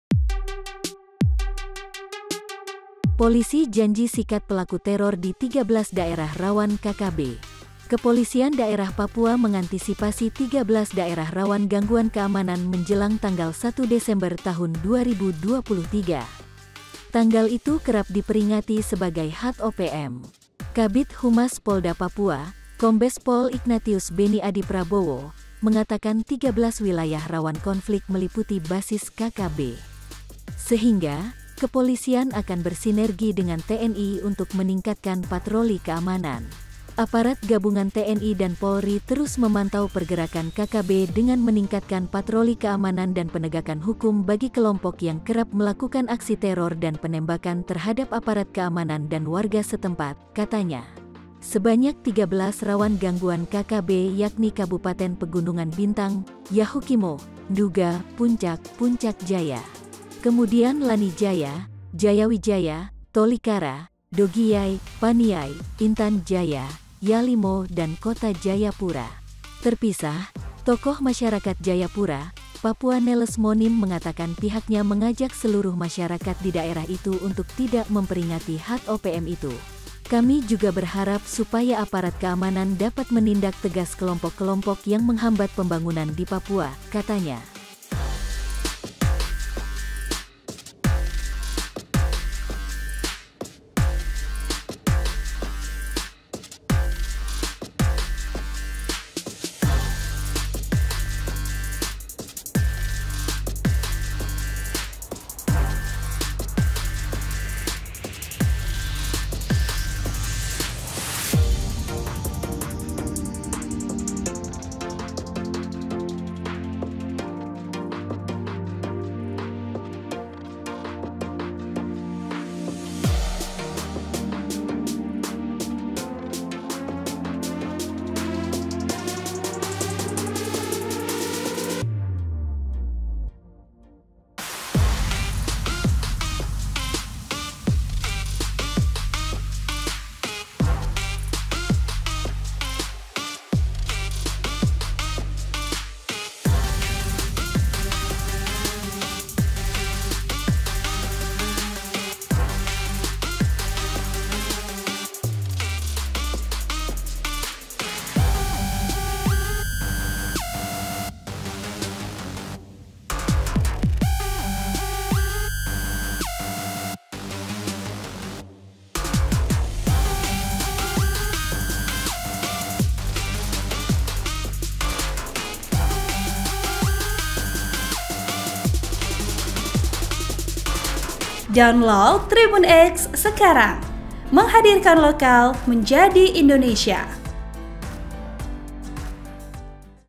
Polisi Janji Sikat Pelaku Teror di 13 Daerah Rawan KKB
POS-KUPANG.COM - Kepolisian Daerah Papua mengantisipasi 13 daerah rawan gangguan keamanan menjelang 1 Desember 2023. Tanggal itu kerap diperingati sebagai HUT OPM.

Kabid Humas Polda Papua, Kombes Pol Ignatius Benny Adi Prabowo, mengatakan 13 wilayah rawan konflik meliputi basis KKB. Sehingga, kepolisian akan bersinergi dengan TNI untuk meningkatkan patroli keamanan.

"Aparat gabungan TNI dan Polri terus memantau pergerakan KKB dengan meningkatkan patroli keamanan dan penegakan hukum bagi kelompok yang kerap melakukan aksi teror dan penembakan terhadap aparat keamanan dan warga setempat," katanya.

Sebanyak 13 rawan gangguan KKB yakni Kabupaten Pegunungan Bintang, Yahukimo, Nduga, Puncak, Puncak Jaya.

Kemudian  Lanny Jaya, Jayawijaya, Tolikara, Dogiyai, Paniai, Intan Jaya, Yalimo dan Kota Jayapura.

Terpisah, Tokoh Masyarakat Jayapura, Papua Neles Monim mengatakan pihaknya mengajak seluruh masyarakat di daerah itu untuk tidak memperingati HUT OPM itu.

"Kami juga berharap supaya aparat keamanan dapat menindak tegas kelompok -kelompok yang menghambat pembangunan di Papua," katanya.
(POS-KUPANG.COM-Lenny)